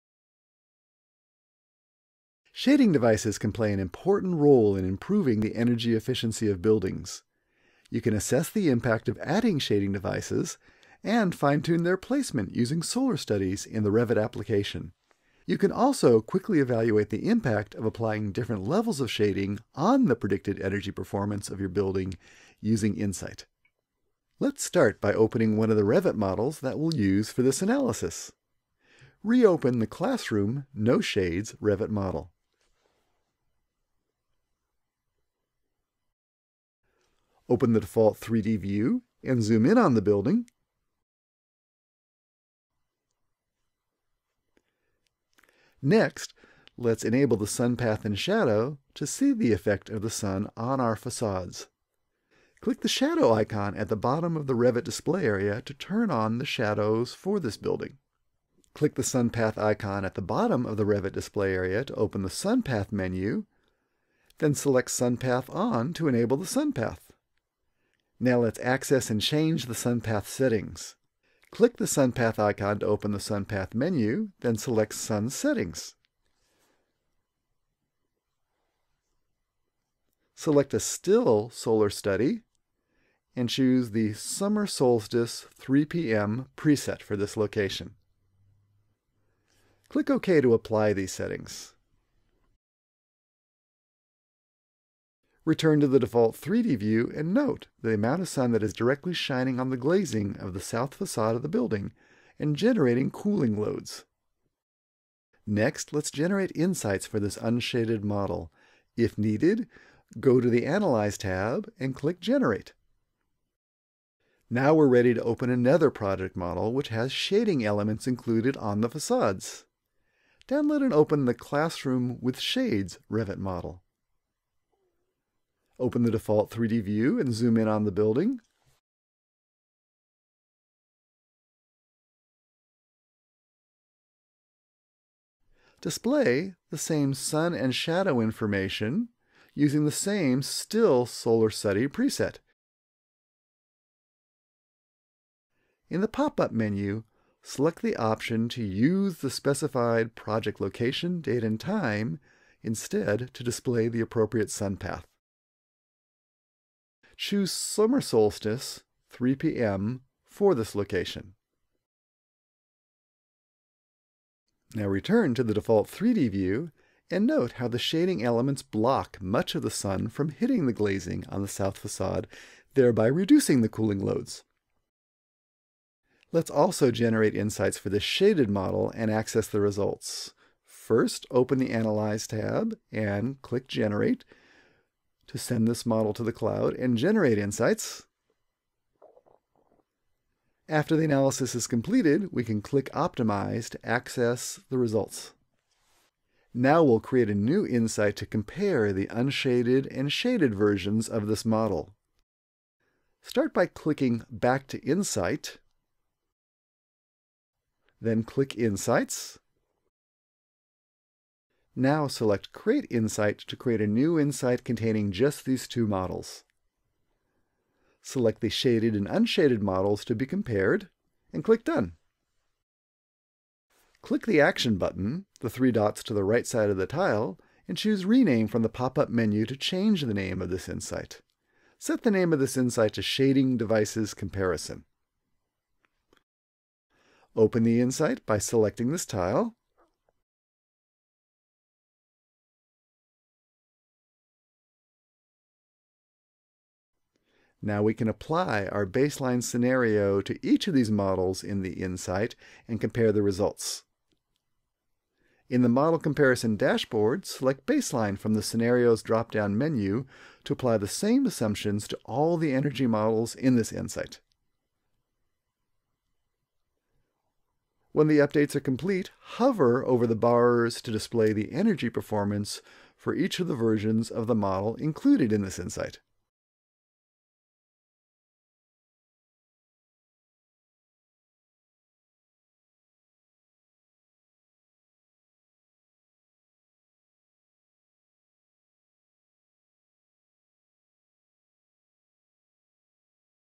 Shading Devices and Energy Consumption
Workflow Steps:
Open the Project with No Shading Elements
• Reopen the Classroom-NoShades Revit model.
Enable Sun Path and Shadow
• Open the default 3D view and zoom in on the building.
• Click the Shadow icon at the bottom of the Revit display area to turn on the shadows for this building.
• Click Sun Path icon at the bottom of the Revit display area to open the Sun Path menu.
• Select Sun Path On to enable the Sun Path
• In the pop-up window, select the option Use the specified project location, date, and time instead to display the Sun Path.
Access and Change Sun Path Settings
• Click the Sun Path icon to open the Sun Path menu
• Select Sun Settings
• To create the desired type of solar study, perform the following changes:
• Select a Still Solar Studies
• Choose the Summer Solstice – 3pm preset.
• Click OK to apply settings.
• Note the amount of sun that is directly shining on the glazing of the South façade of the building and generating cooling loads.
Generate Insights for this Unshaded Model
• If needed, generate insights for this unshaded model.
Open Another Project with Shading Elements Included
• Download and open the Classroom-WithShades Revit model.
• Display Sun and Shadow information for the same Still Solar Study preset – Summer Solstice – 3pm preset.
• Return to the default 3D view.
• Note how the shading elements block much of the sun from hitting the glazing on the South façade, thereby reducing the cooling loads.
Generate Insights for the Shaded Model and Access Results
• Generate insights.
• Click Optimize to access the results.
Create a New Insight to Compare the Unshaded and Shaded Models
• Click Back to Insight
• Click Insights
• Select Create Insight.
• Select the shaded and unshaded models to be compared and click Done.
• Click the action button (three dots to the right side of the tile) and choose Rename from the pop-up menu to change the name of the Insight.
• Set the name of this Insight to Shading Devices Comparison.
• Open the Insight by selecting the tile.
Apply Shared Assumptions within an Insight
• In the Model Comparison Dashboard, select Baseline Scenario from the Scenarios drop-down menu to apply the same assumptions to all the energy models in this Insight.
• Hover over the bars to display the energy performance of each model in this insight.